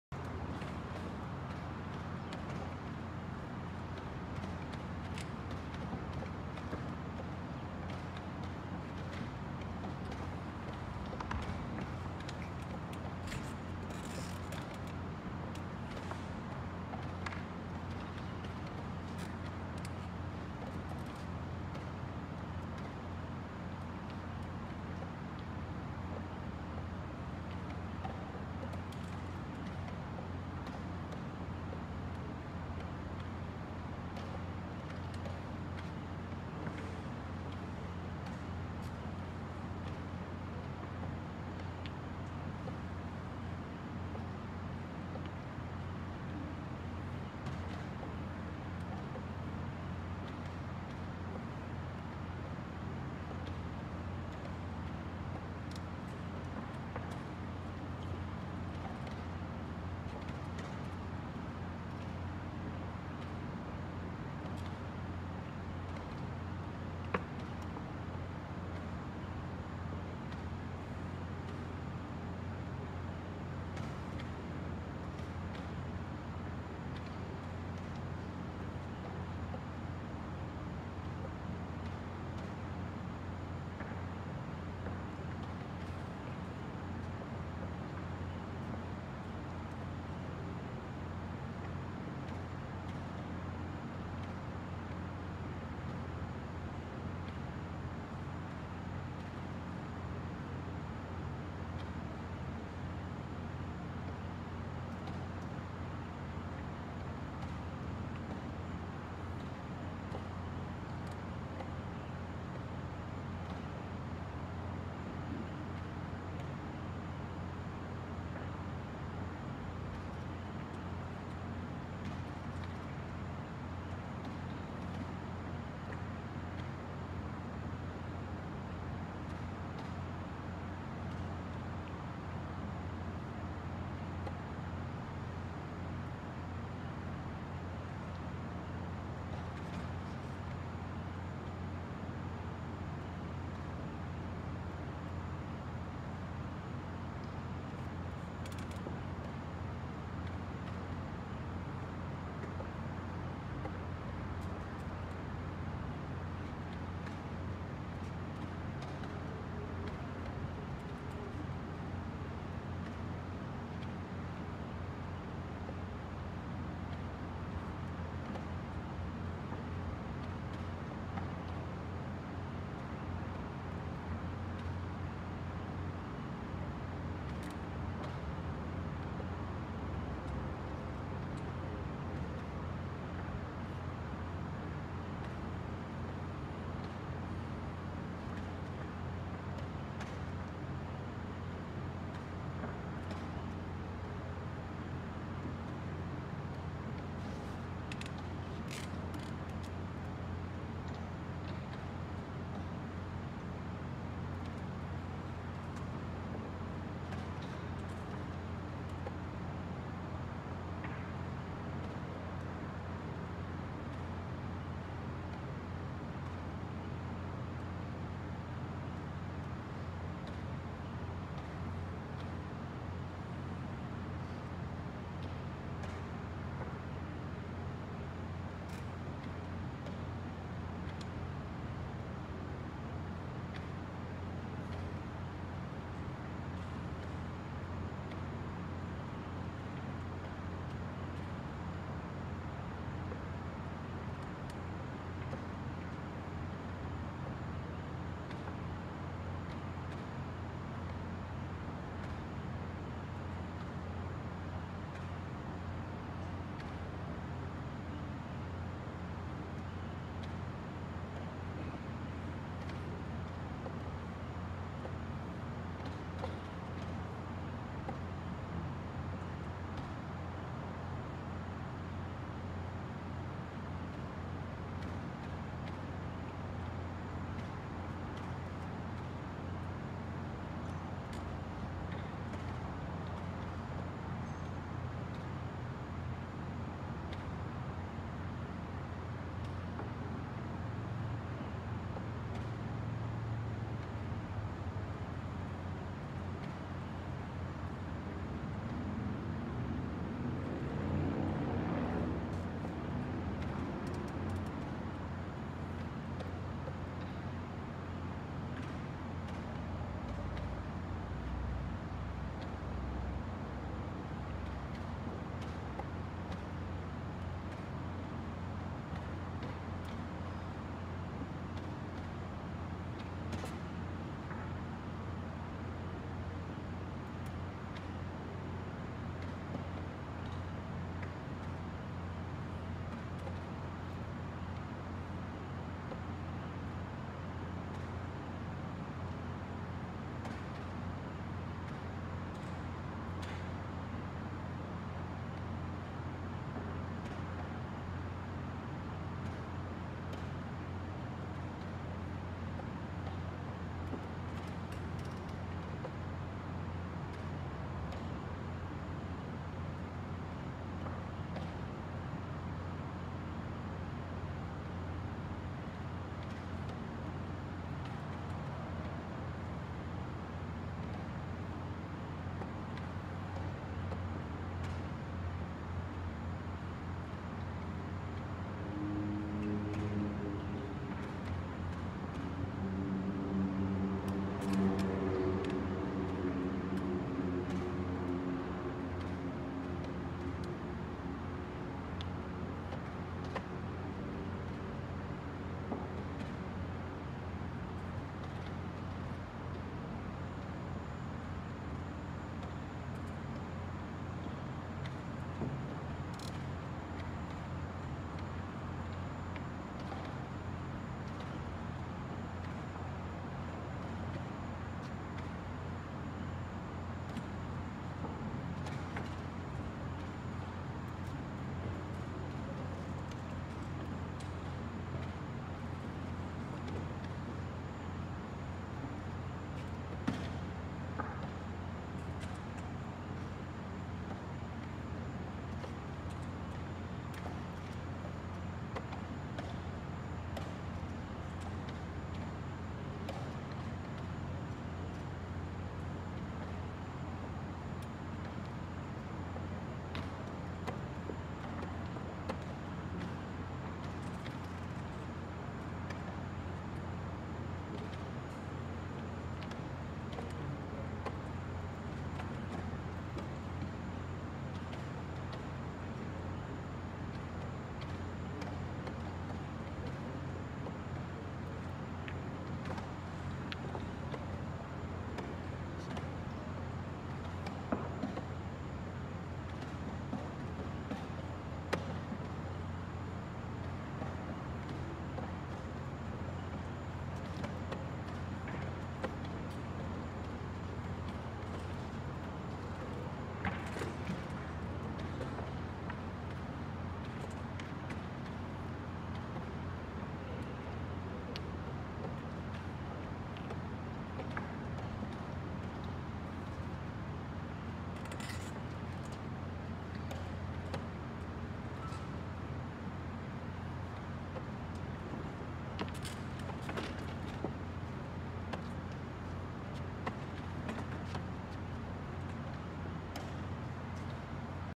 GM Reynaldo Vera (CUB) vs GM Alexander Shabalov (USA) Blitz Chess
GM Reynaldo Vera de Cuba vs GM Alexander Shabalov de Estados Unidos ocurrido durante el Festival de Ajedrez de San Juan llevado a cabo del 21 al 26 de marzo de 2019 en el Natatorium de San Juan.